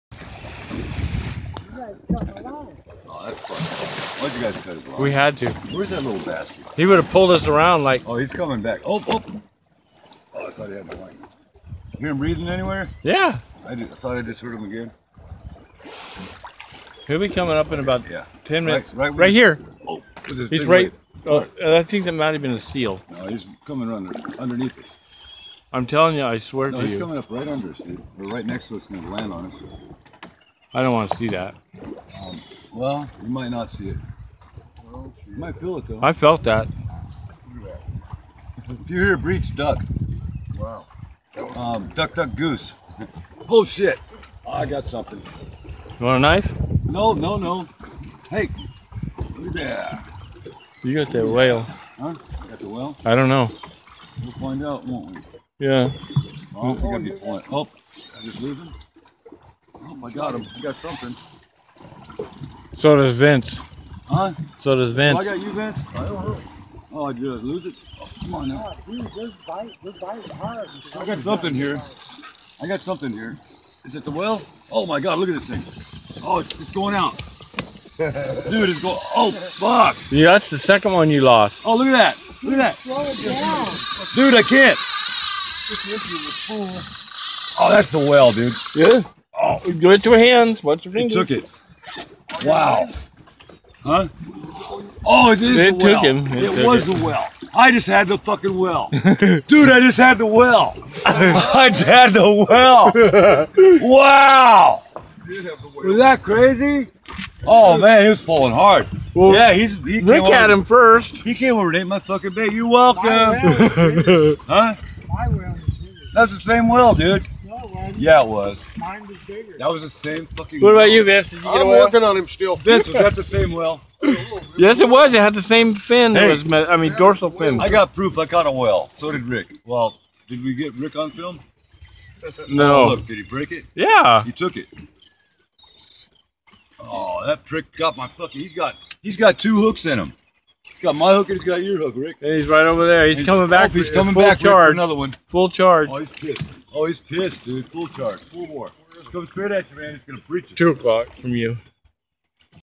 John catching "Moby's Dick"
Morro Bay rock-cod fishing in John Robinsons boat with Daniel Souza, Vince Kinney, Rick Tackit@ 125'
1 of 2 Videos